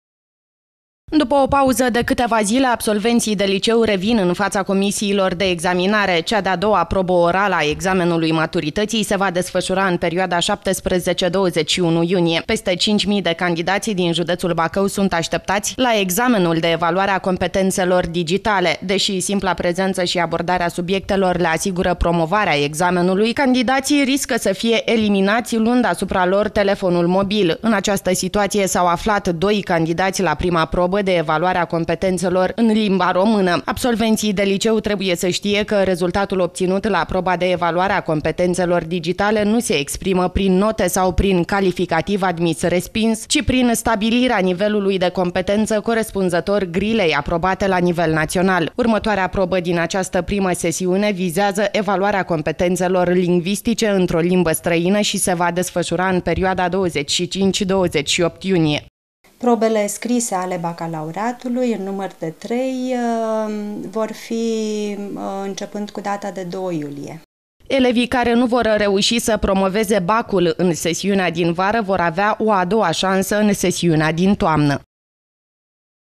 Incepe cea de-a doua proba a BAC-ului - (16.06.2013)
Sesiunea de vara a Bacalaureatului continua cu proba de evaluare  a competenţelor  digitale. La aceasta proba telefonul mobil reprezinta cel mai mare pericol pentru candidati.